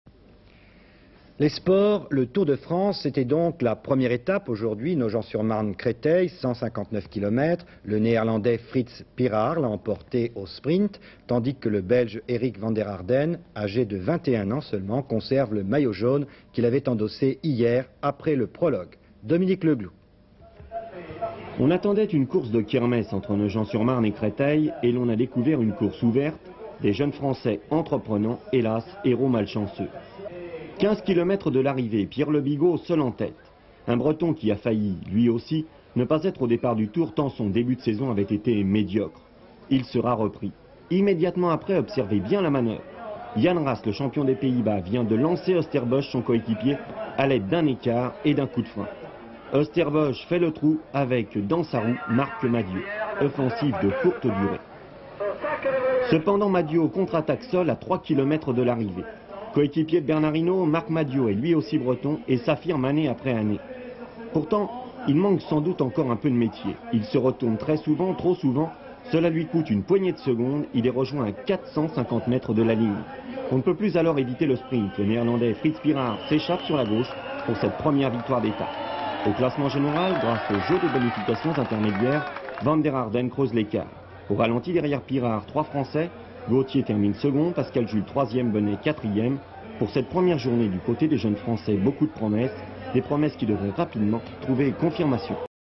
Tour de France : 1ère étape
2 juillet 1983
 Arrivée de la 1ère étape du tour de France 1983, entre Nogent-sur-Marne et Créteil. Le Néerlandais Frits PIRARD remporte l'étape, tandis que le Belge Eric VANDERAERDEN conserve le maillot jaune, endossé après le prologue. Commentaire sur images de la course avec Pierre LE BIGAULT doublé par Bert OOSTERBOSCH, la contre attaque de Marc MADIOT, le sprint final et l'arrivée de Frits PIRARD. Images d'archive INA